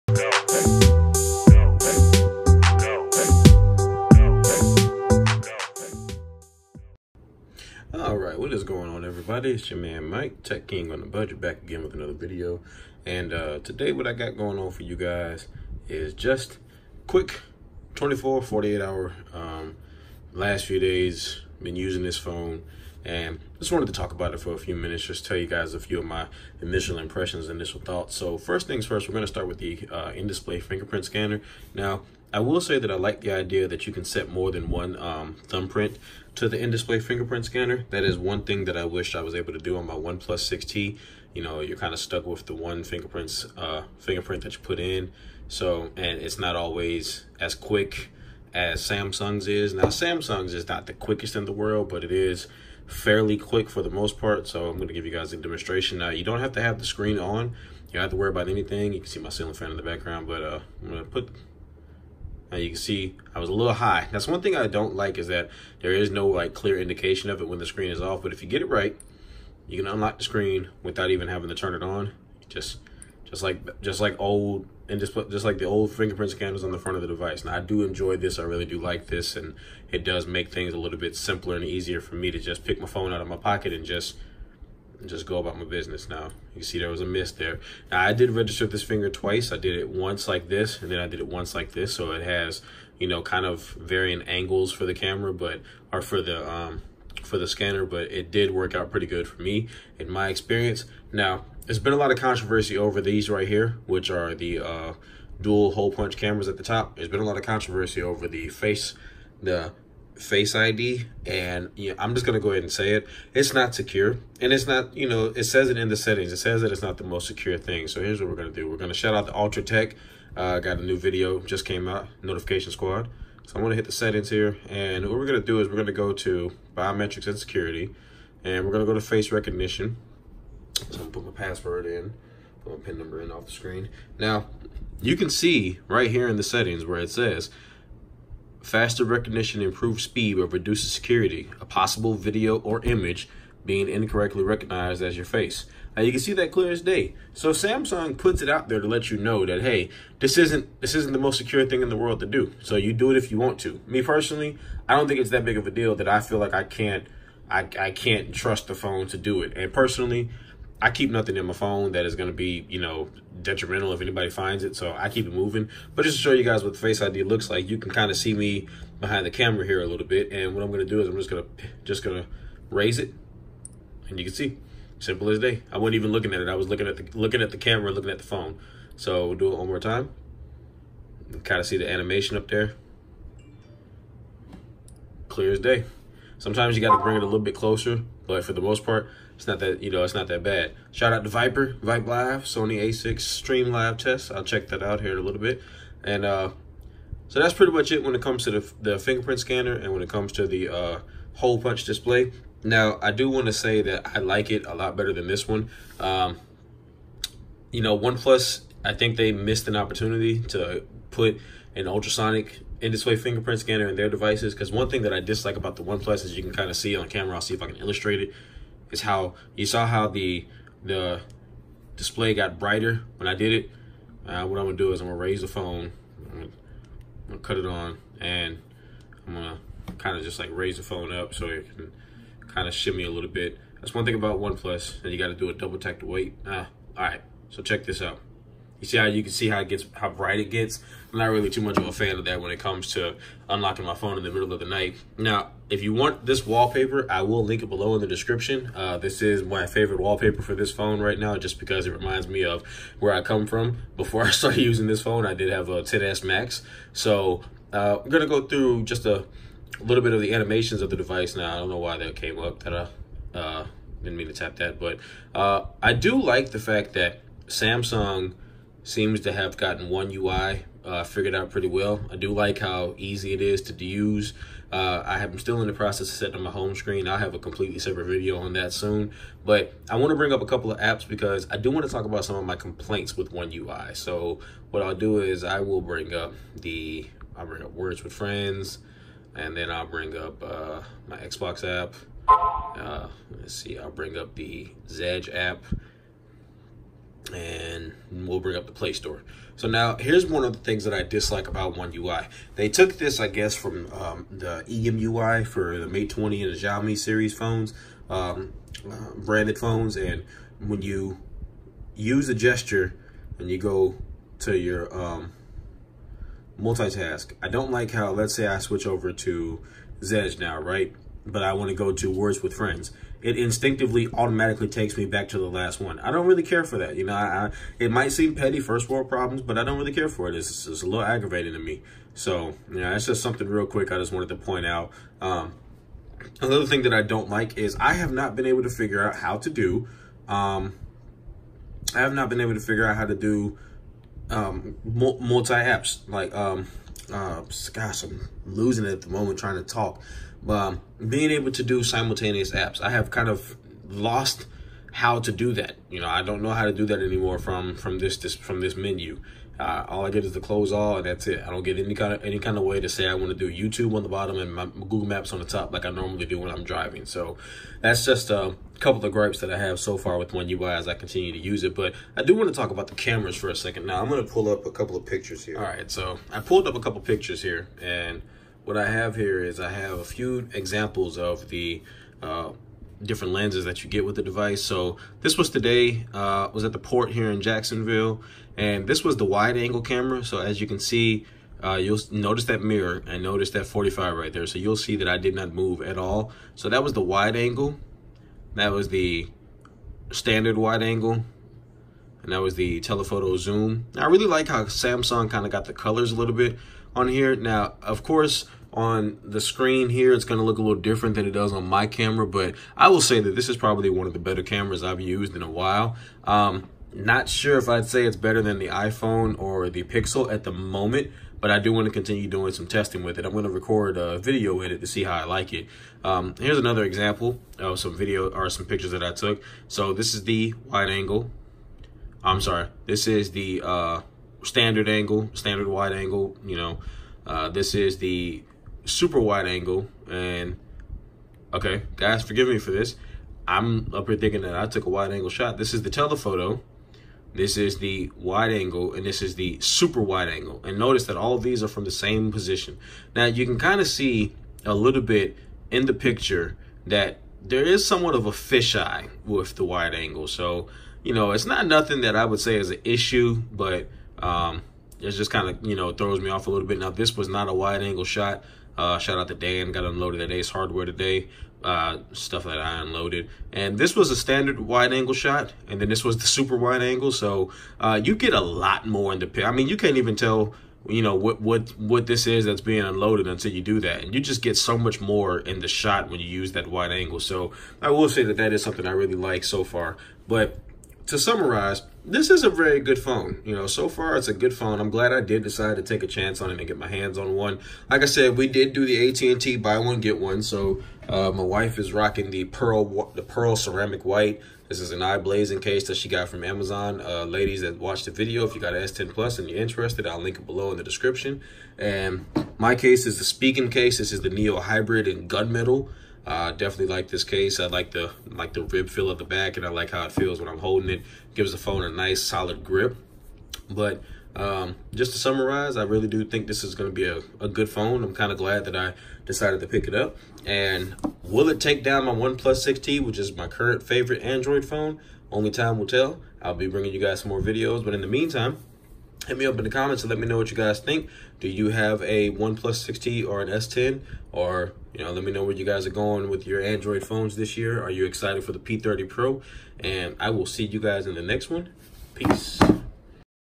48 HOURS WITH THE GALAXY S10+
Just a quick update after using the S10+ for a few days.
Main thing taking away so far is the cameras! Look for a video on those cameras soon!

Like the music? Hit up my producer directly everywhere @popfontaine